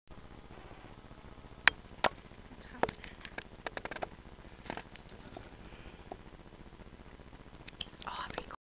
002- Accidentally Filming.
We were posing for a picture, but our neighbor accidentally had the camera on the video setting. Kind of a lame video, but I could not bring myself to delete it, since Hadley is in it. Video made on March 19, 2012, Monday- Hadley is just under one week old.